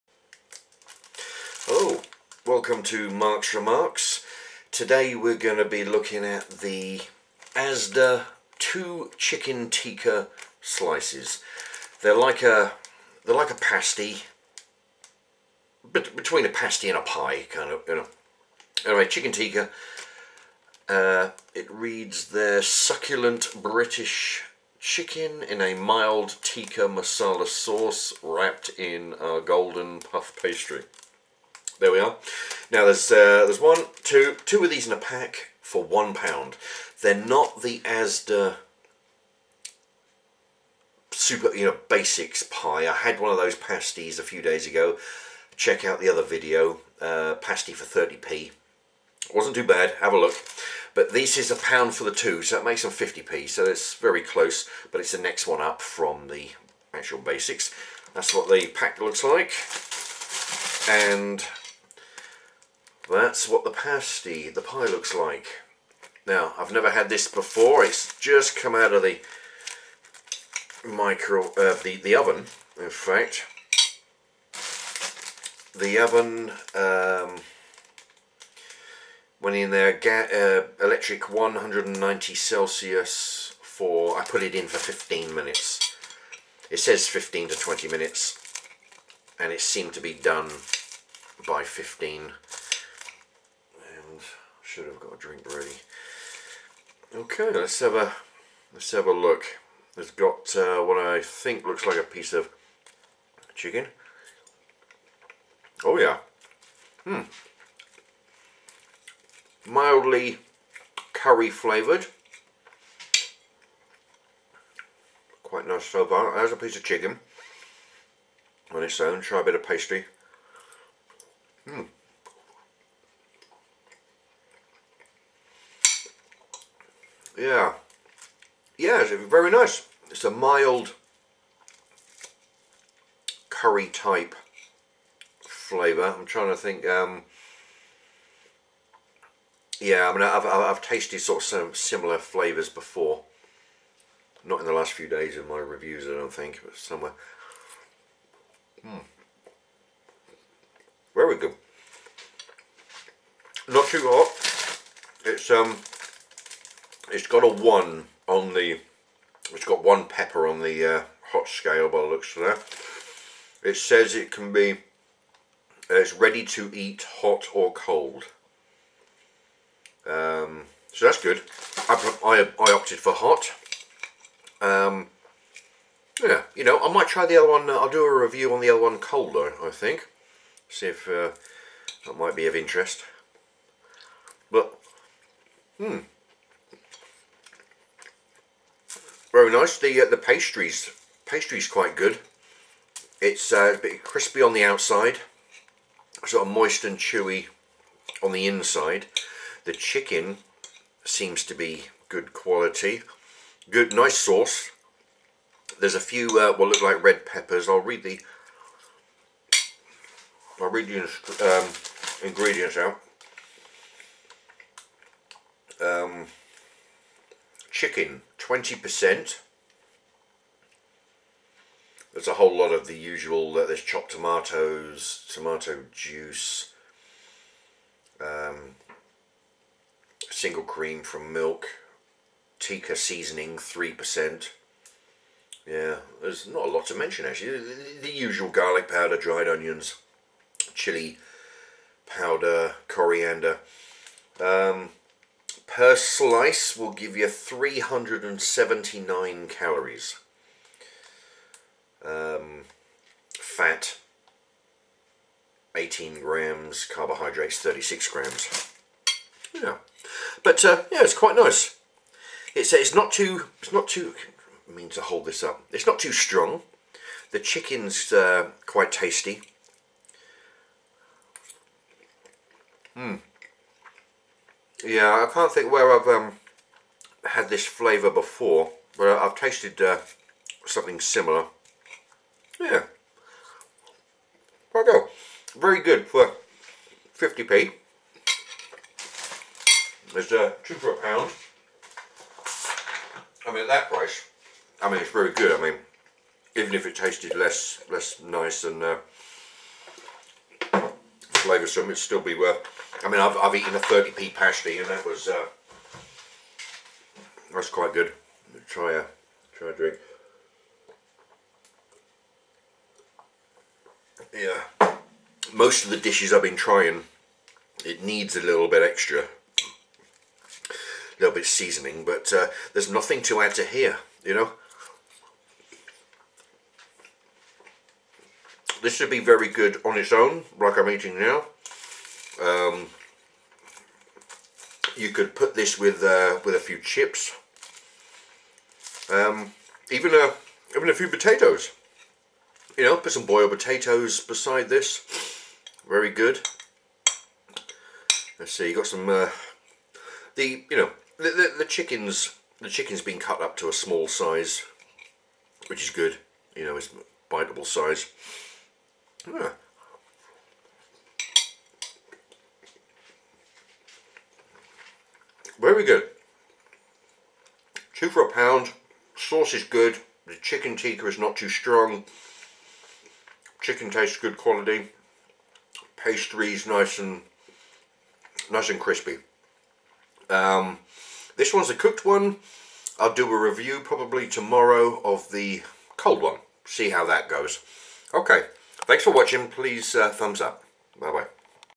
marks REmarks Asda 2 chicken tikka slices (eaten hot) review
pretty good tikka flavour, 2 slices for £1, good value.